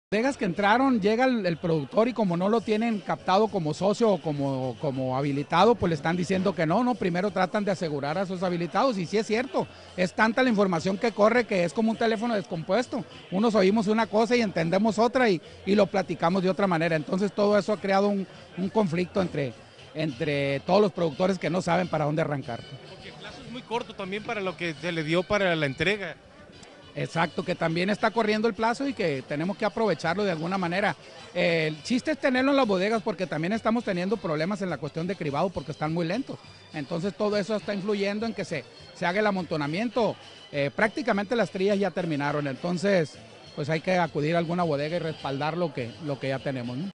Piden mayor claridad para el acopio de las 50 mil toneladas de frijol
La Coordinadora Única de Productores Agrícolas está pidiendo mayor claridad en las reglas publicadas por Aserca para el acopio de las 50 mil toneladas de frijol dentro del programa para esperar un repunte en el precio de la leguminosa.

Emilio González señaló que existe mucha desinformación en ese sentido, pues las bodegas están priorizando a sus habilitados para almacenar su grano y dejan en lista de espera al resto de los productores.